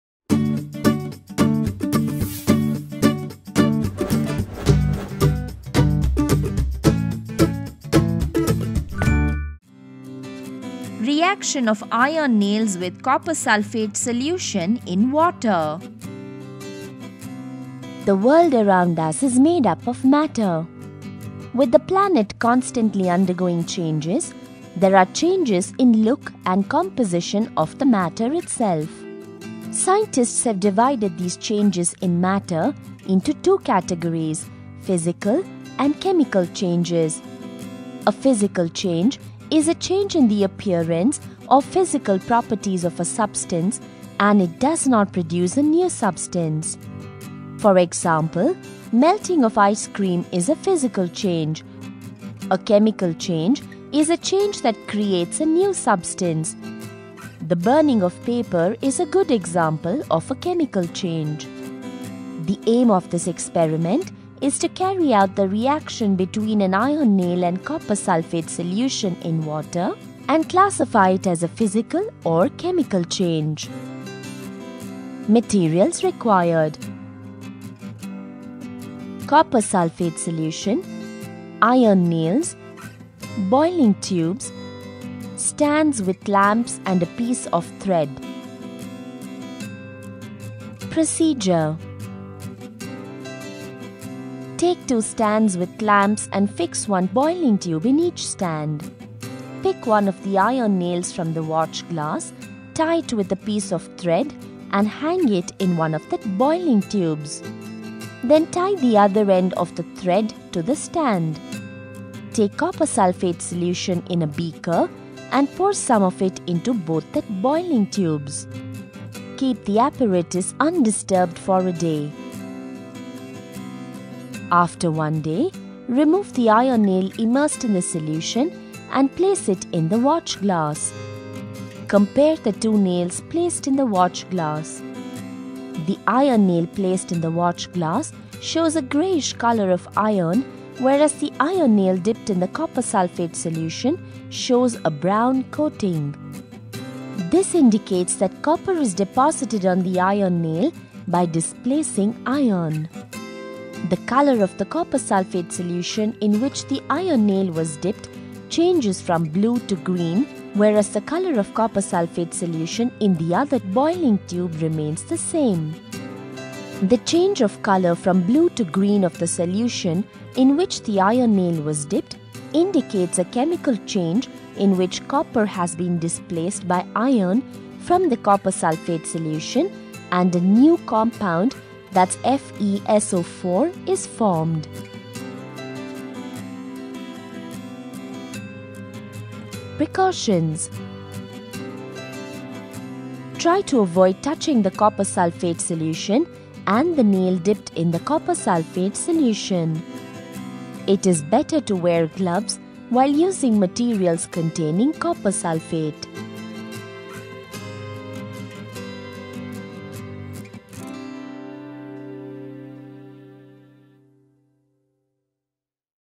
Reaction of Iron Nails with Copper Sulphate Solution in Water - MeitY OLabs
▶ Online Labs for School lab Experiments (Olabs)

Developed by CDAC Mumbai & Amrita University under research grant from Department of IT, Government of India

Reaction of Iron Nails with Copper Sulphate Solution in Water :-
When an iron nail is dipped in copper sulphate solution, a brown coating of copper is formed on the surface of iron and the colour of copper sulphate solution changes from blue to light green. This reaction shows that iron is more reactive than copper as it displaces copper from its solution and iron passes into solution as Fe (II) ions and ferrous sulphate solution is formed.

This video explains the chemical change occurs during the reaction of iron nails with copper sulphate solution.